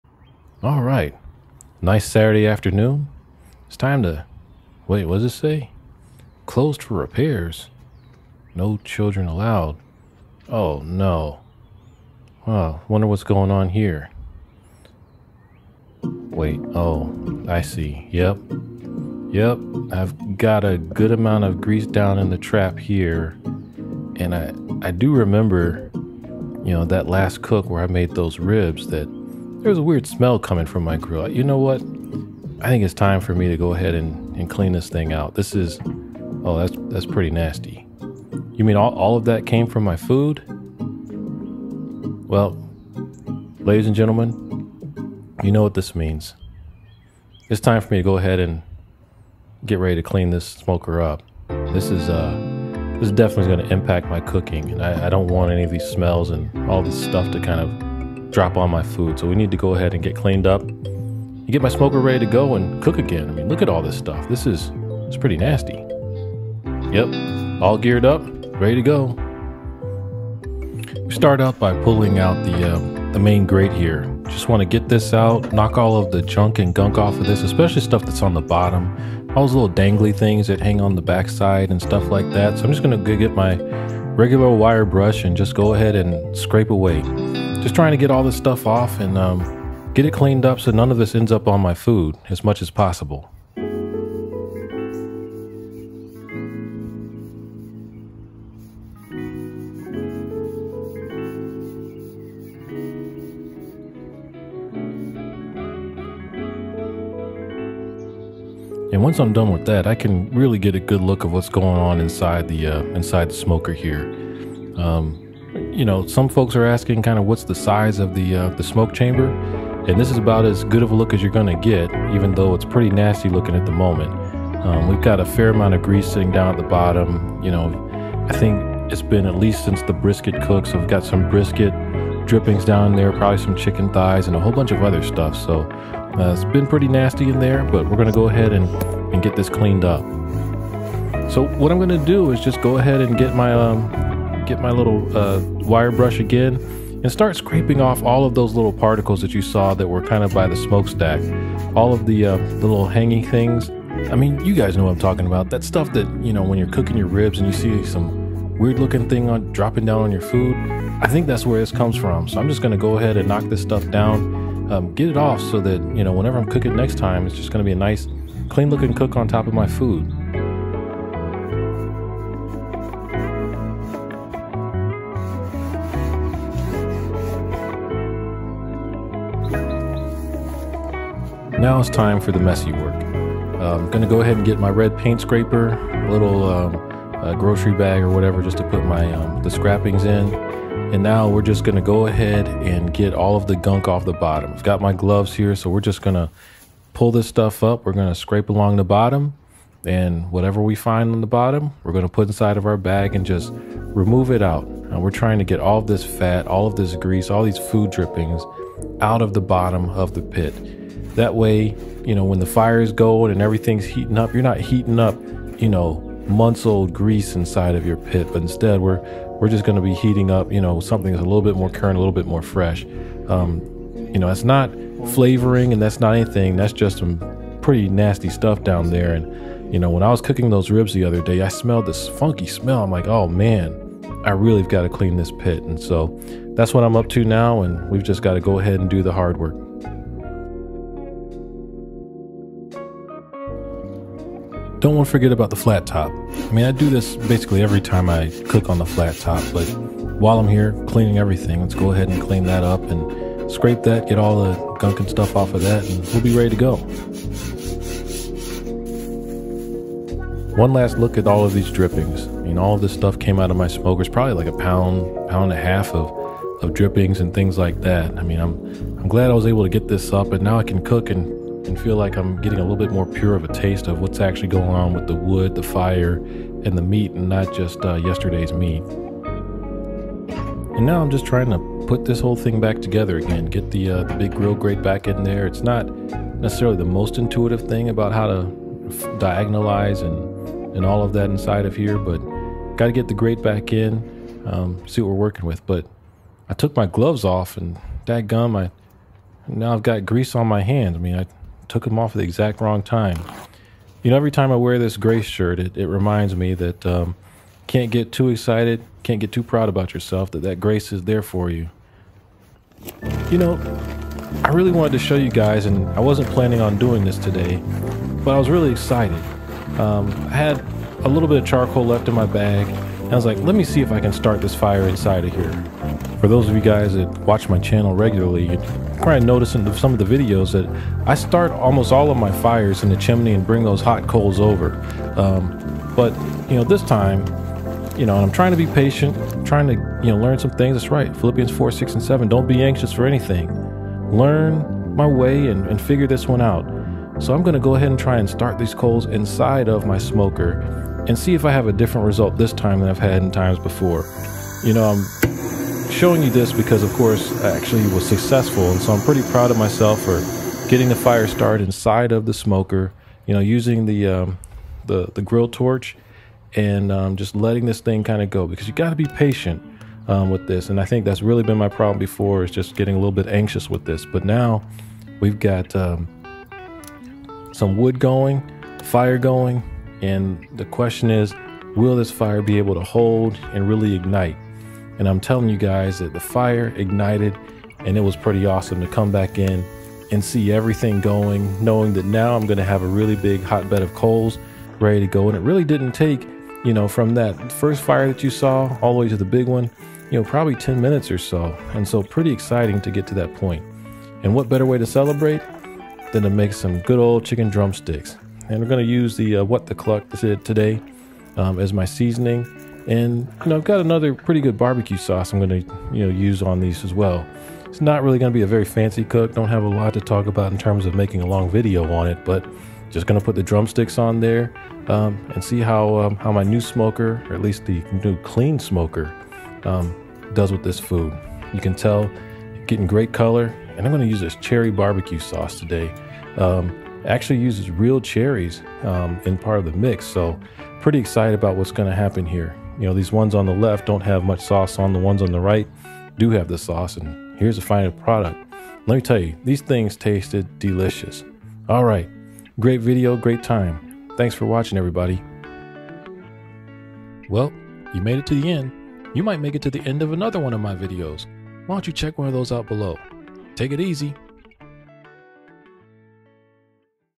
Cleaning the Mill Scale 94 Gallon *Bonus Smoked Chicken Drumsticks with Cherry Barbecue Sauce
Its time to clean the offset smoker. I am using a wire brush, a paint scraper and elbow grease to get the gunk out of the bottom of the pit. After I finished, I cooked some chicken drumsticks on the smoker.